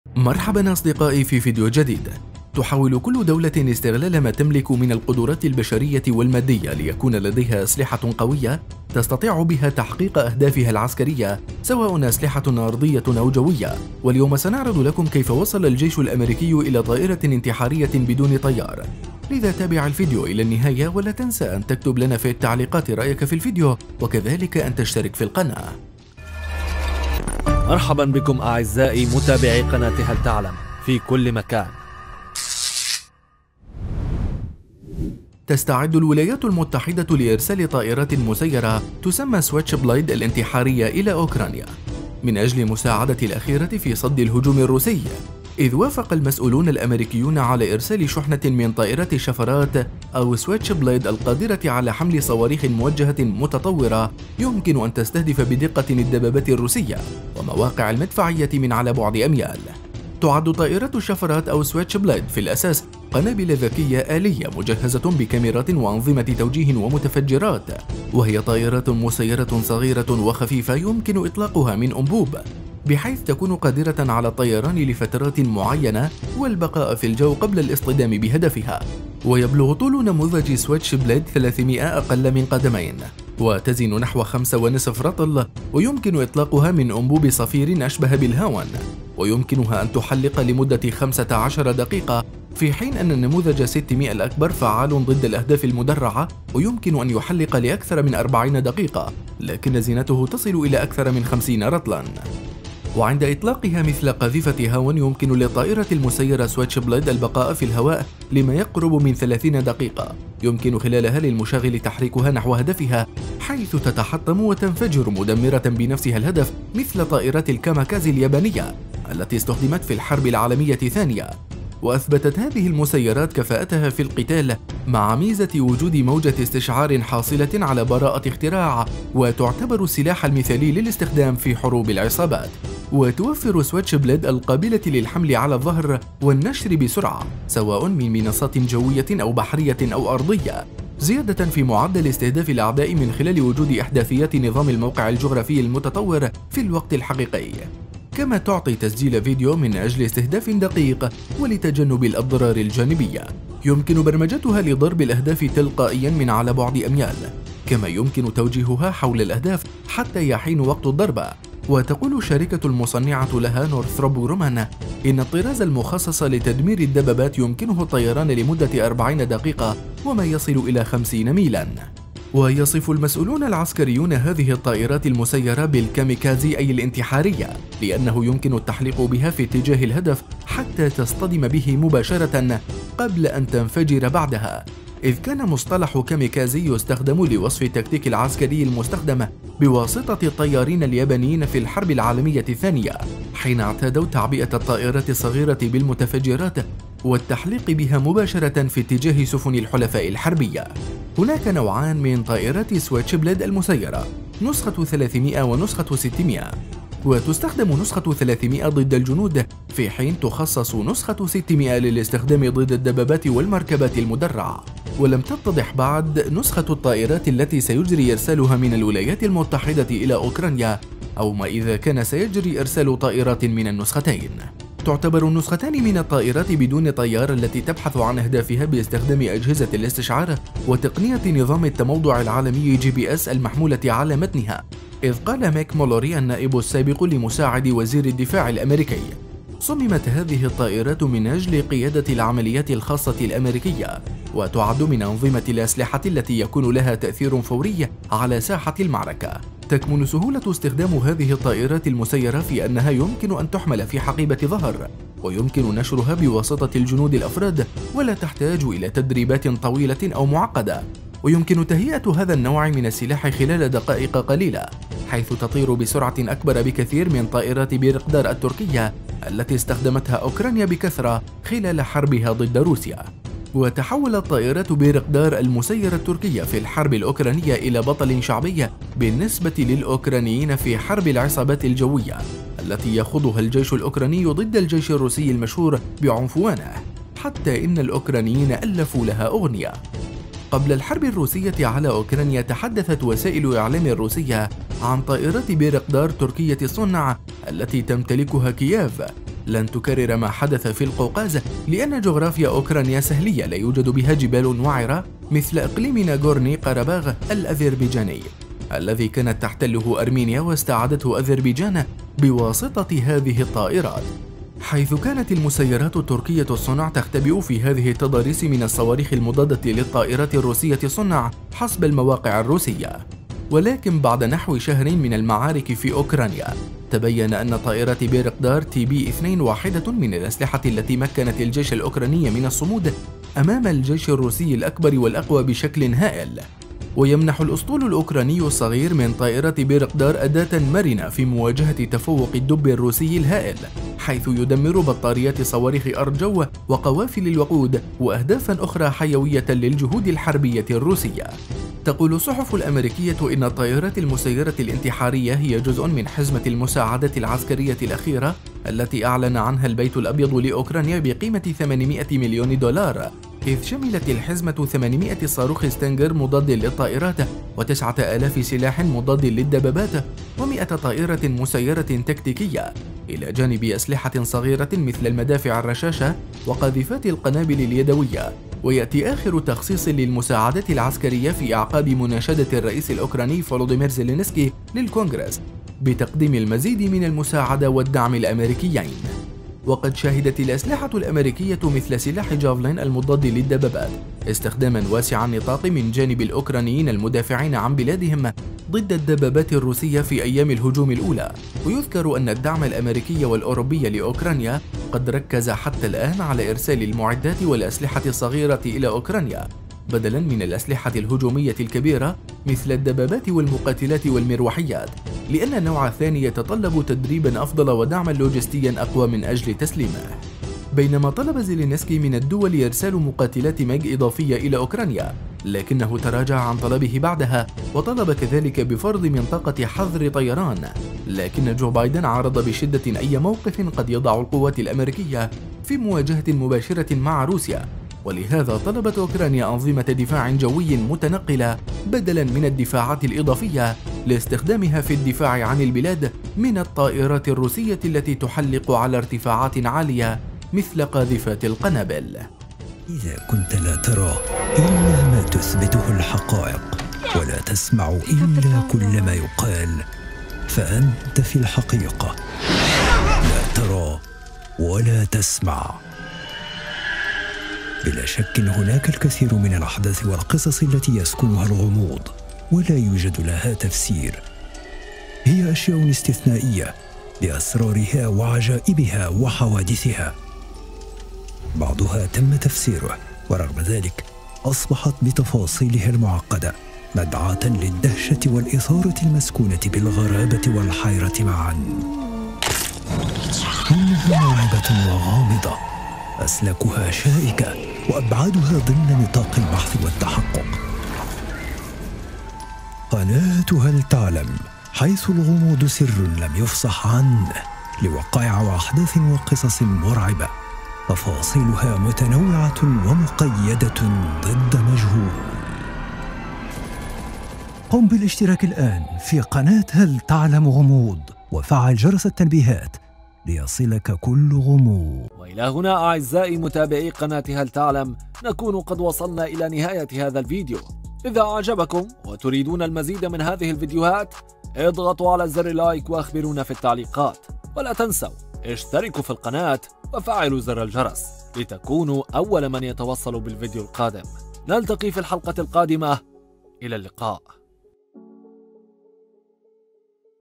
سلاح أمريكي جديد بتقنية مدمرة سينهي حرب روسيا مع اوكرانيا
الاتصال أو الدعم والإعلان مع مجموعة قنوات هل تعلم قم بزيارة الرابط :
مرحبا اصدقائي فى فيديو جديد.. تحاول كل دولة استغلال ماتملك من القدرات البشرية والمادية ليكون لديها اسلحة قوية تستطيع بها تحقيق اهدافها العسكرية سواء اسلحة ارضية او جوية, واليوم سنعرض لكم كيف وصل الجيش الامريكي الى طائرة انتحارية بدون طيار, لذا تابع الفيديو الى النهاية ولا تنسى ان تكتب لنا فى التعليقات رايك فى الفيديو وكذلك ان تشترك فى القناة.